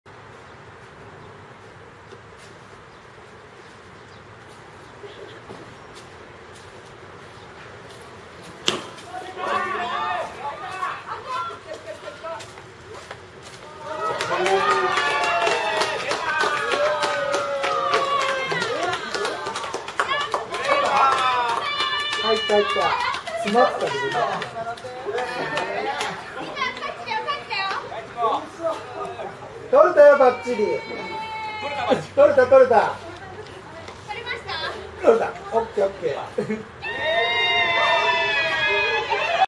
2009-05-21 KSリーグ チャレンジャーズVSチェリーパイ5回裏 忍氏HR HD画質
草薙球場 KSリーグ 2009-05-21 VSチェリーパイ戦 チェリーパイ忍さんホームラン ハイビジョン画質版 撮影機器：SANYO Xacti DMX-HD2000